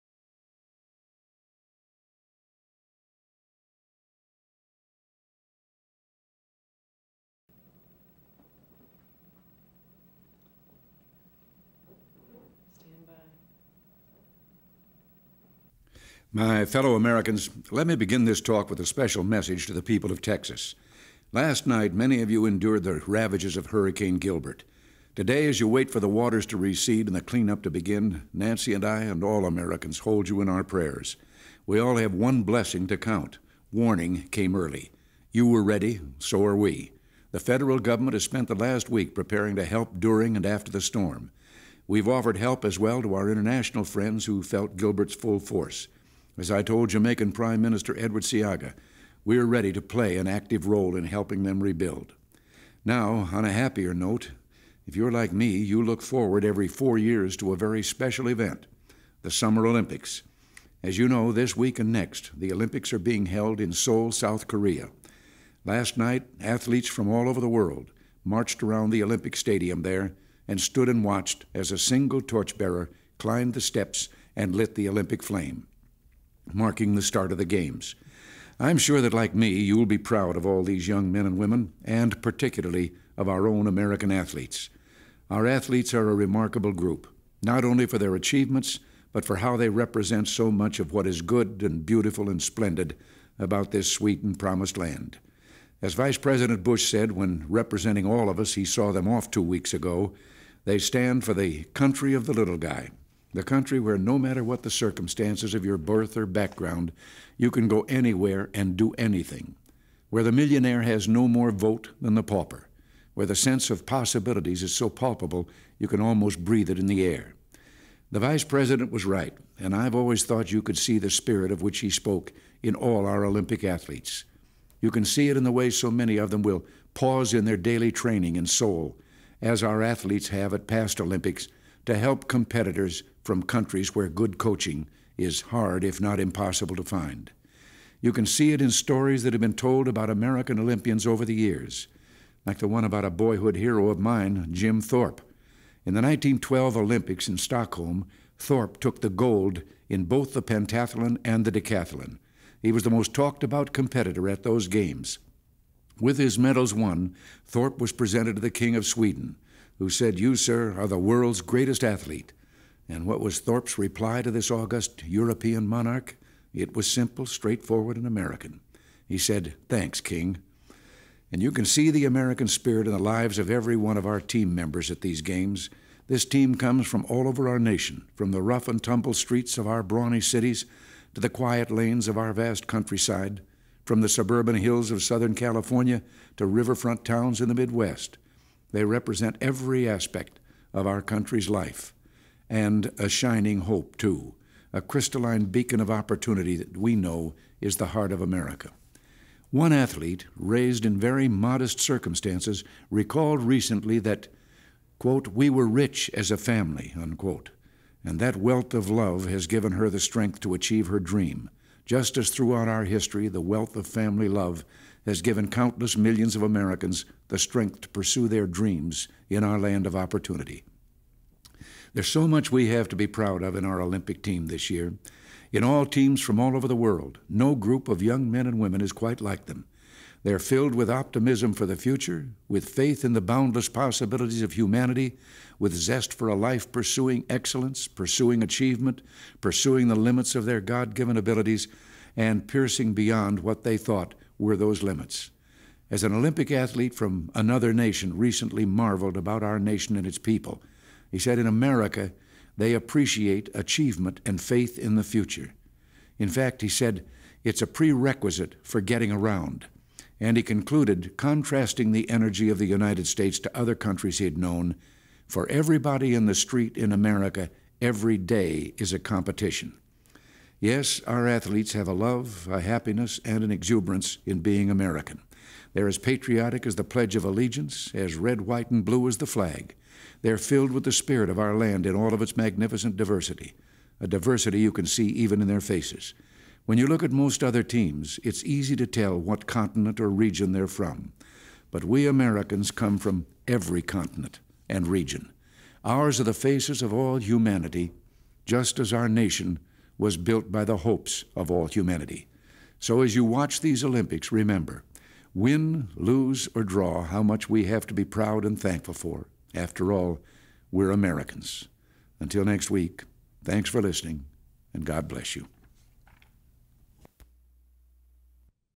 President Reagan's Radio Address on the Summer Olympics on September 17, 1988
Full Title: President Reagan's Radio Address to the Nation on the Summer Olympics from Camp David, Maryland on September 17, 1988

Access Restriction(s):Unrestricted
Use Restriction(s):Unrestricted